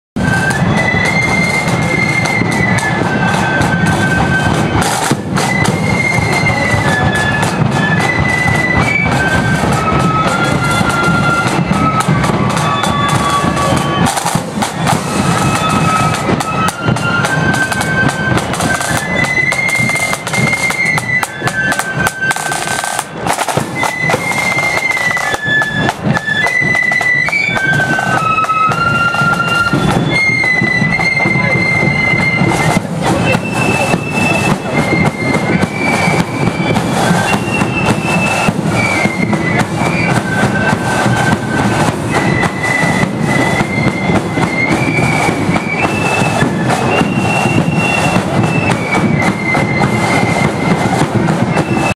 Blackskull Orange & Blue(2) @ Pride of Knockmore 09/05/2014
Blackskull Orange & Blue On the March in Lisburn for Pride of Knockmore annual parade